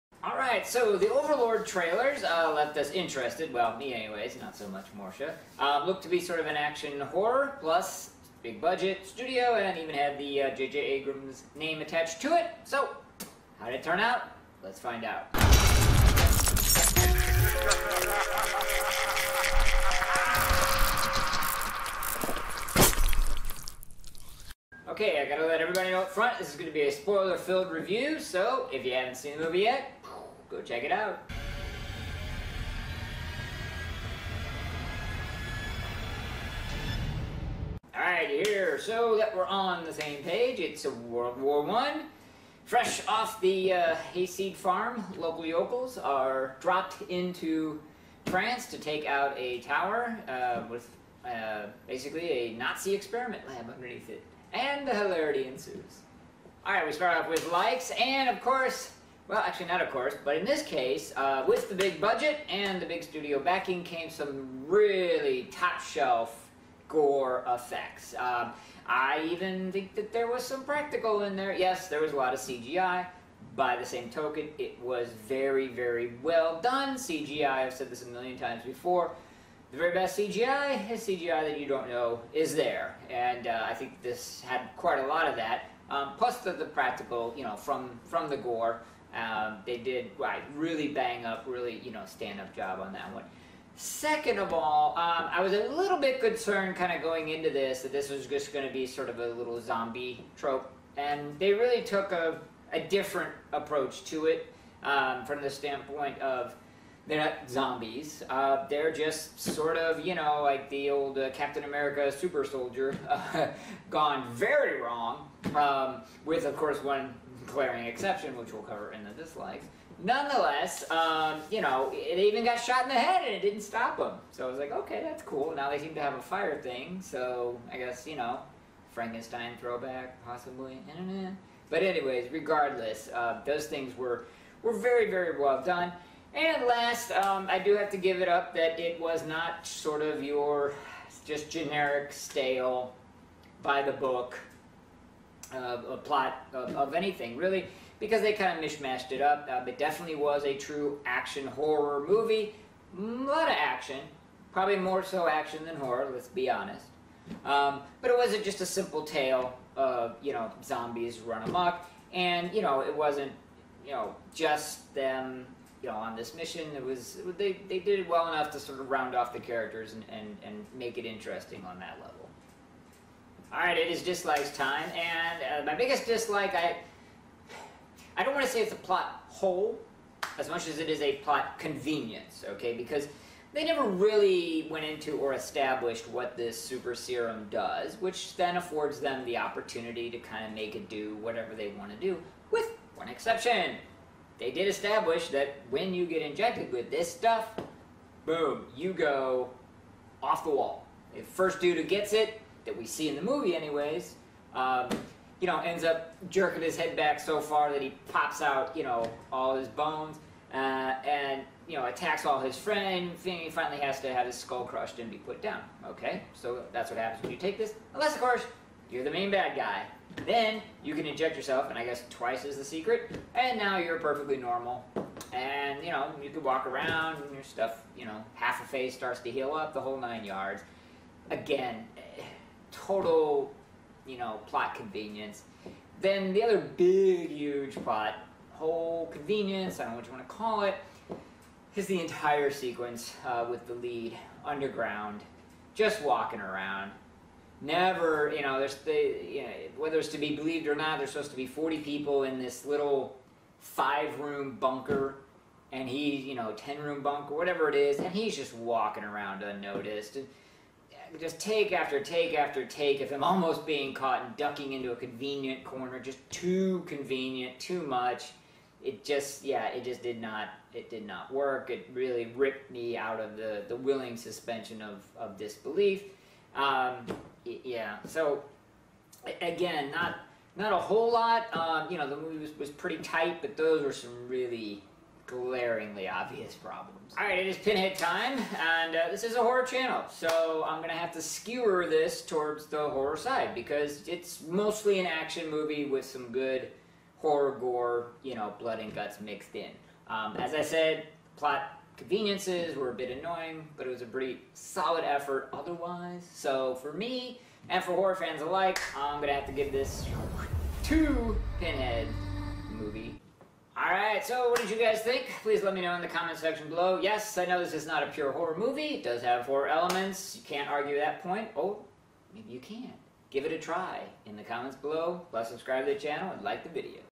Overlord Spoiler Review
In our trailer reactions, we expressed cautious optimism and needed clarification that this was most likely an action movie with shades of horror. Mortia didn't want to see it at all so Dante braved it alone - the result? Find out in our Overlord Spoiler Review!!!

Director: Julius Avery
Writers: Billy Ray, Mark L. Smith
Stars: Jovan Adepo, Wyatt Russell, Mathilde Ollivier